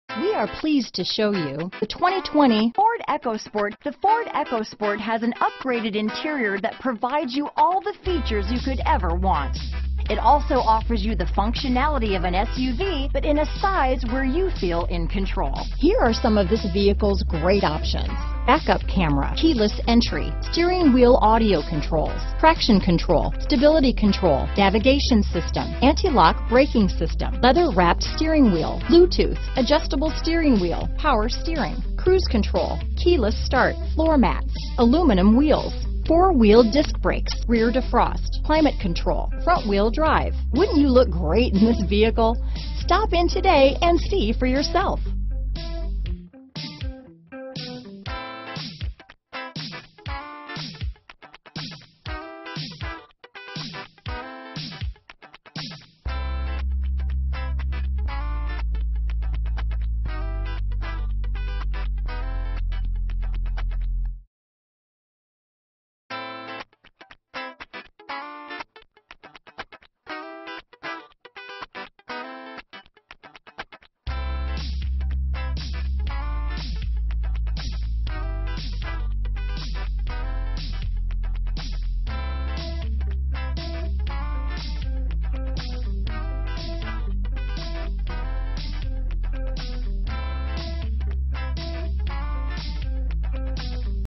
2020 Ford EcoSport The Villages, Leesburg, Belleview, Wildwood, Crystal River, FL 200381
Moondust Silver Metallic New 2020 Ford EcoSport available in The Villages, Florida at

Village Ford
11660 SE Highway 441

Moondust 2020 Ford EcoSport SE FWD 6-Speed Automatic EcoBoost 1.0L I3 GTDi DOHC Turbocharged VCTRecent Arrival! 27/29 City/Highway MPG